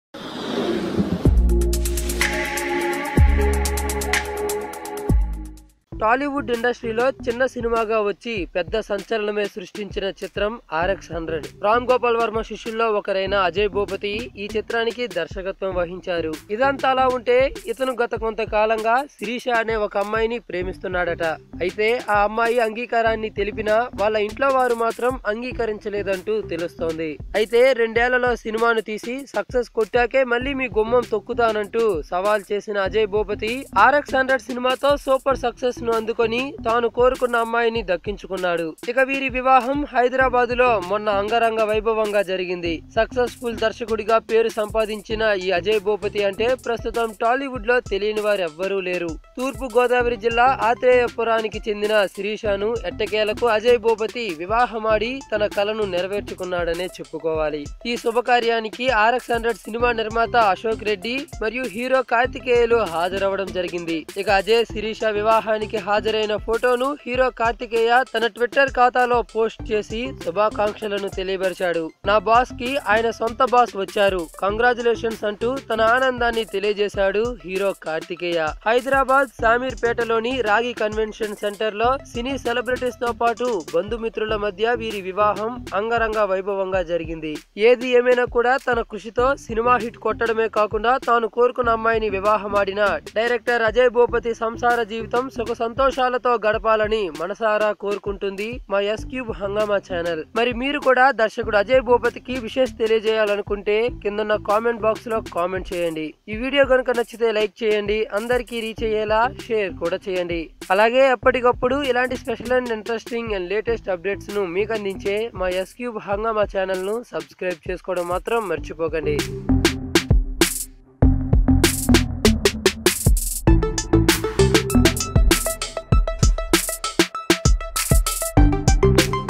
తన లవర్ కోసం RX 100 సినిమాను తీసిన అజయ్ భూపతి చివరికి ఎవరిని పెళ్లాడదో చూడండి |Ajay Bhupathi RX100
తన లవర్  కోసం RX 100 సినిమాను  తీసిన అజయ్ భూపతి చివరికి ఎవరిని పెళ్లాడదో చూడండి |Ajay Bhupathi marriage photos RX100